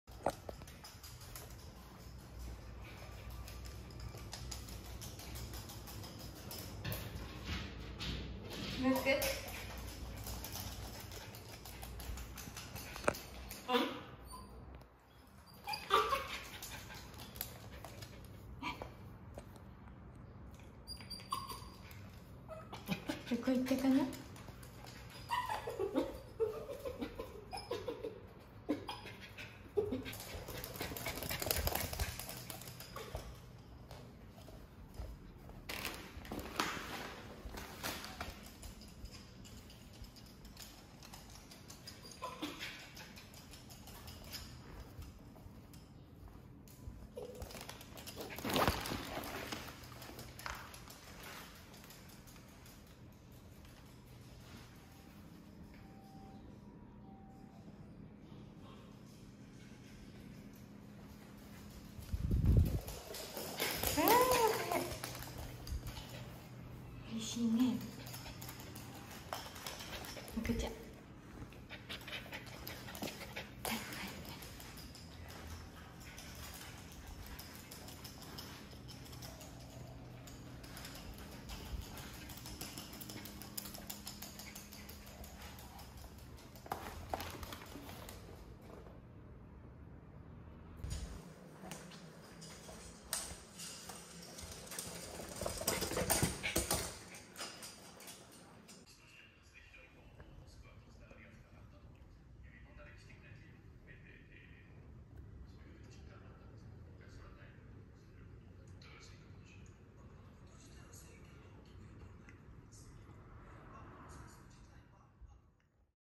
ムクが何をしても良いようにマイホームへ引っ越す
この度は引っ越すことになりました。以前は都会でしたが、少しだけ田舎へ。

周りにも公園や自然が多くムクお嬢様がのびのび出来る所です。